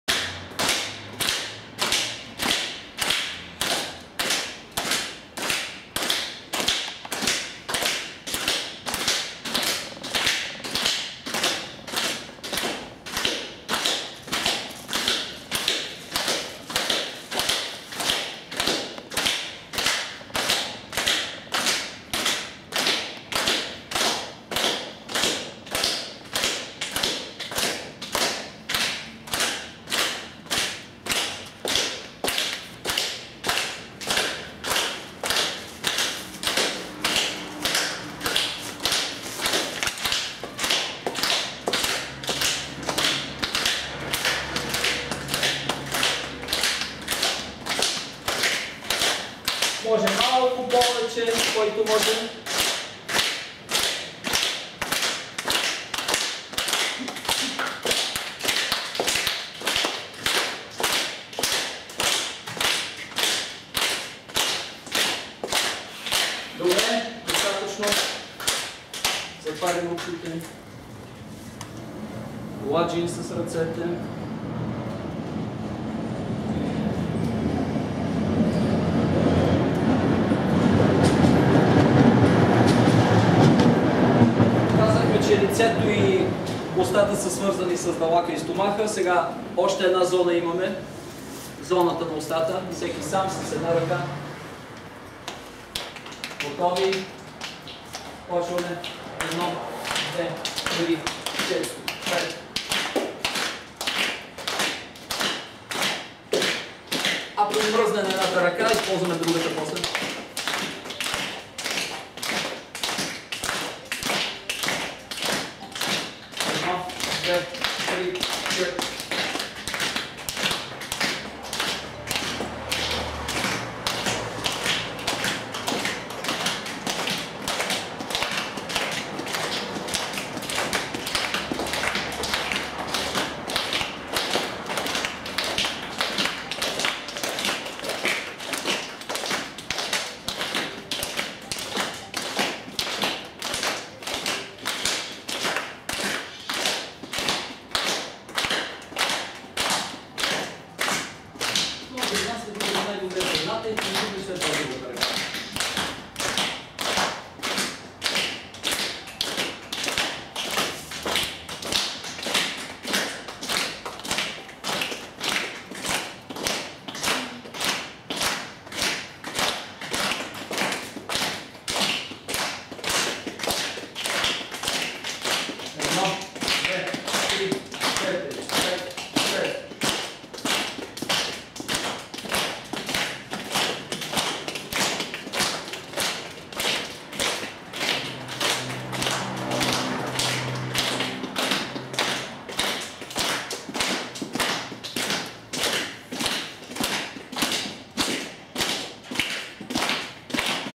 ПАЙДА в зона на очи и уста от семинара в София 13.08.16
Благодаря на всички участници. Българите ще са скоро най-здравата и осъзната нация. Силата на колективния дух е уникална. Предстоящи събития